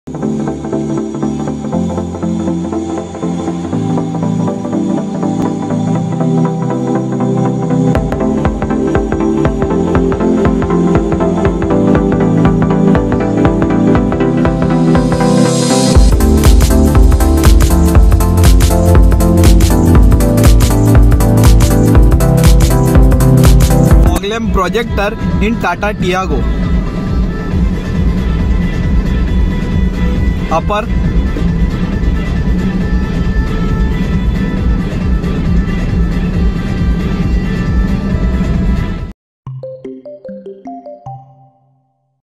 Fog Light ka BAAP😳.Projector FOG LIGHT In Tata TIAGO/Shah Car Decor.
Projector Fog Light Fitted in Tata Tiago with Upper Dipper
At Shah Car Decor.
Surat.